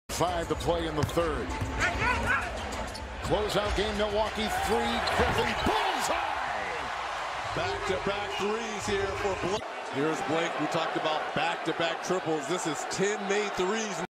NBA, playoff 2019, Bucks vs. Pistons, Round 1, Game 4, Move 47, Blake Griffin, 3 pointer
Milwaukee Bucks - Detroit Pistons, Game 4, 22/04/2019, quarter 3
Blake Griffin, 3 pointer